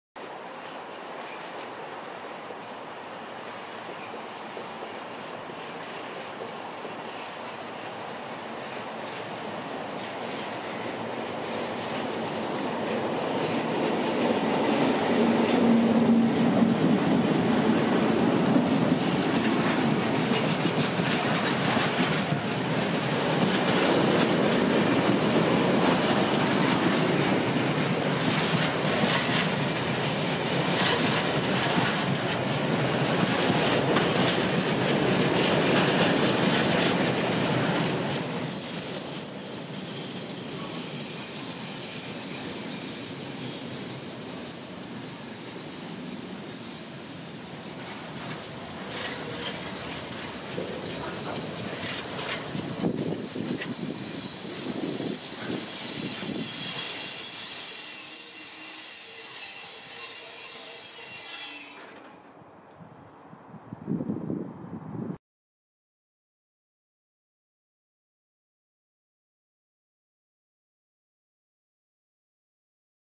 DB Schenker 60001
DB Schenker 60001 rumbles into the DGL @ Bristol Parkway with the Murco Tanks.